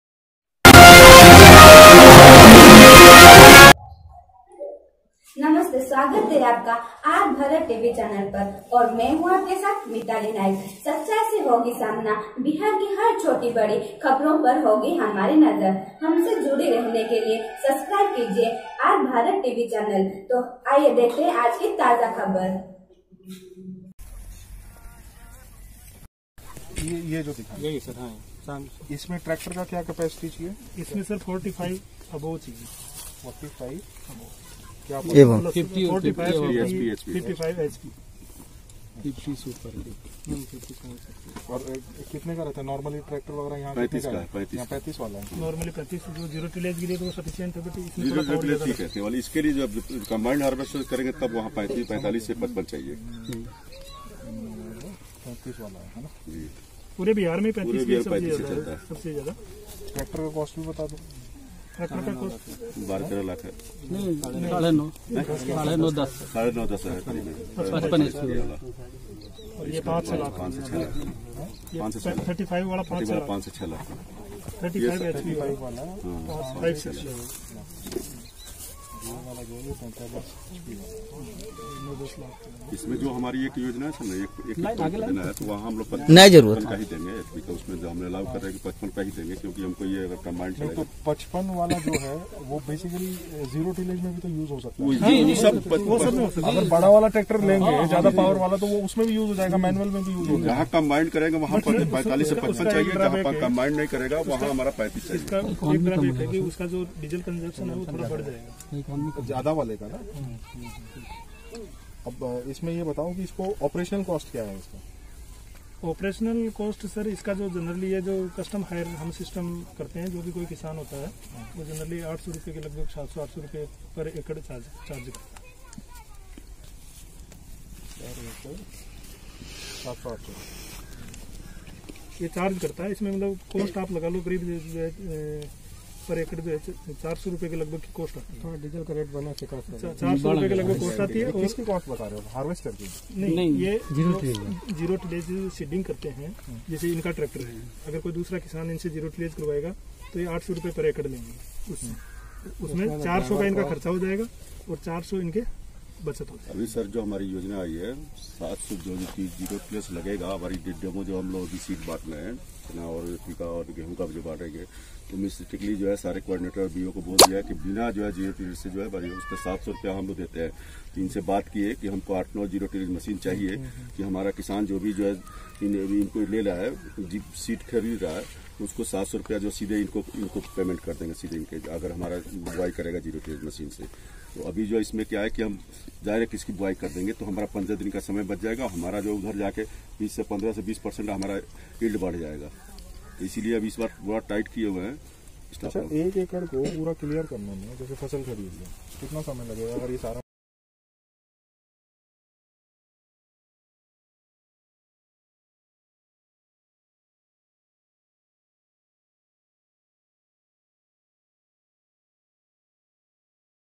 नवादा dm द्वारा कजिया नामक गांव में नए बेलर मशीन और हैप्पी सीडर का डेमो किया गया
R. BHARATA TV